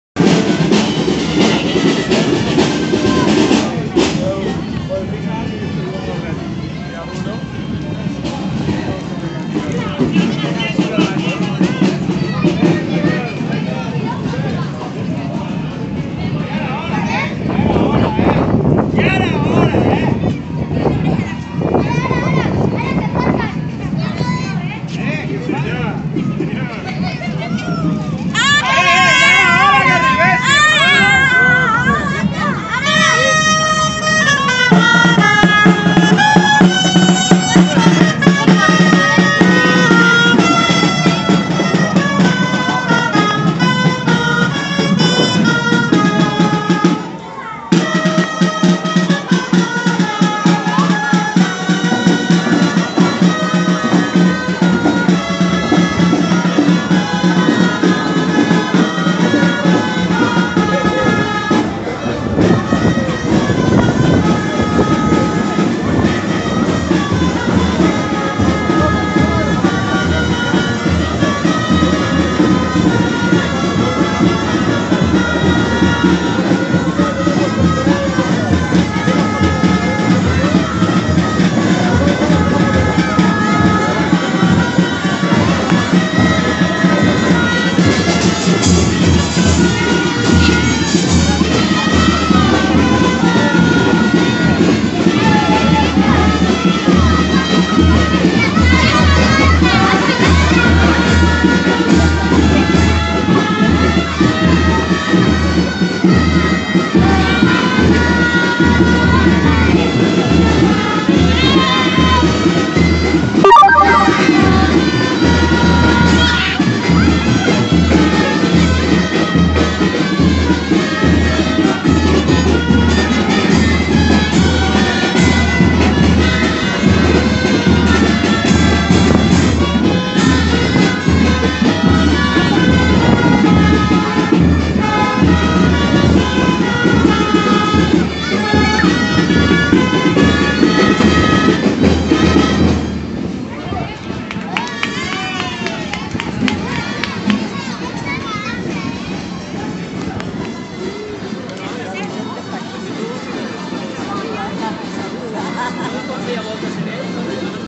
Trobada de gegants del Vallés a Parets- 29-3-2014- passejada
Passejada amb els gegants de Sant Cugat del Vallés, Gegants del Tros de Solsona i del Carrer de la Riera de Vic